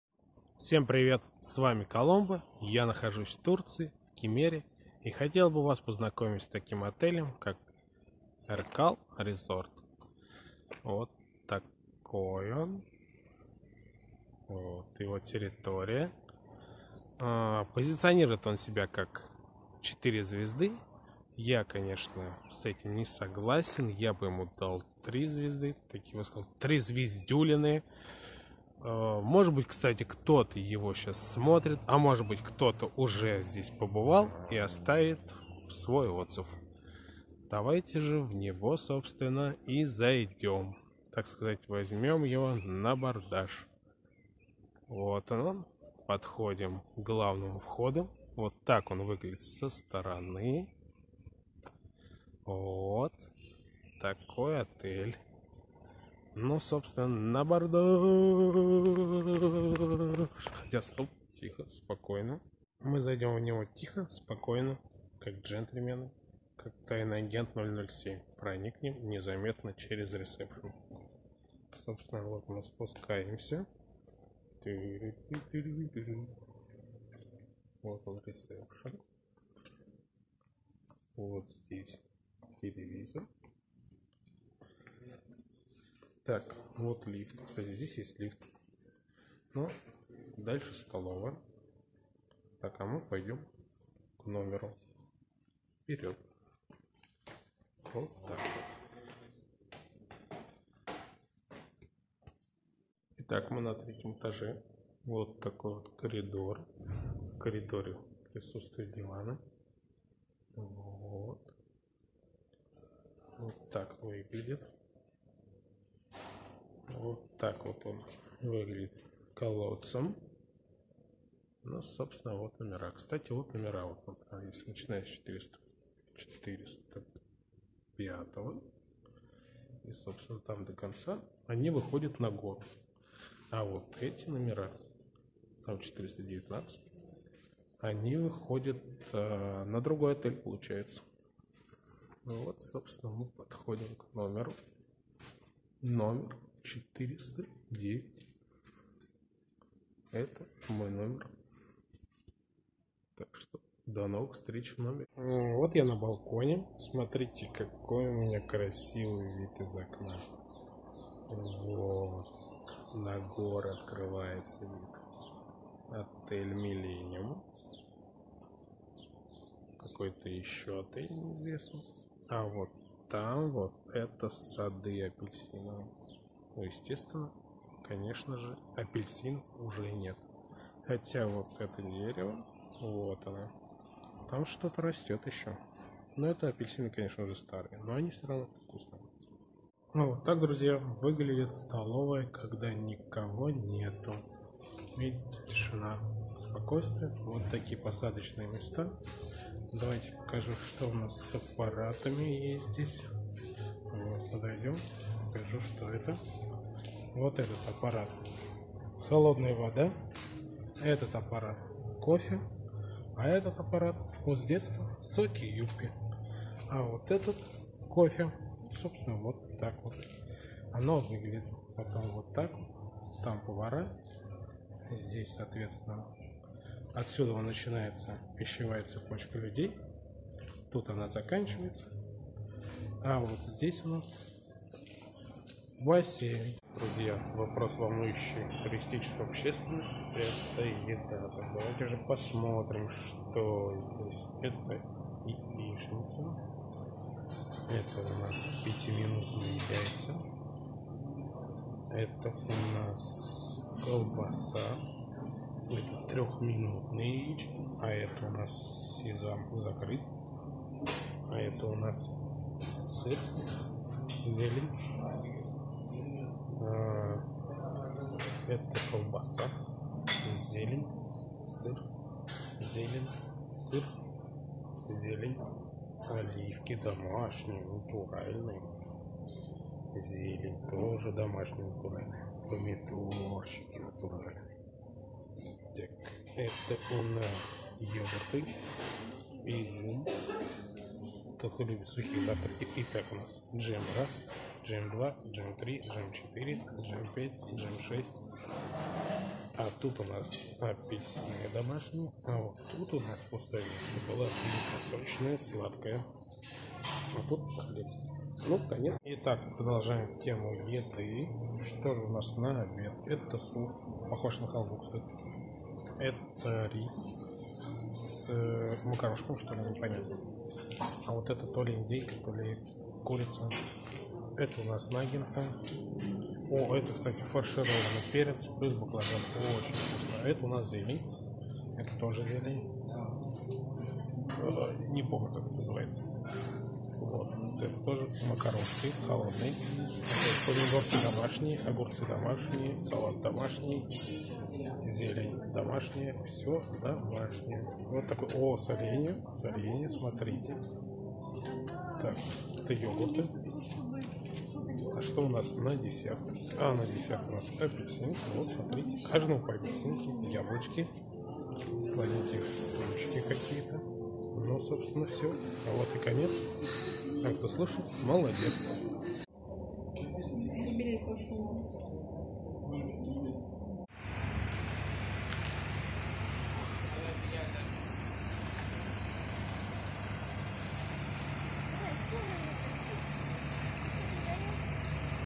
Обзор отеля Erkal Resort Hotel 4* (Турция,Кемер)
Обзор отеля Erkal Resort Hotel 4*, Кемер, Турция. Отдых в Турции, обзор отеля.В отеле загорелась баня и произошёл пожар.Так же в видео будет обзор чем кормят все включено.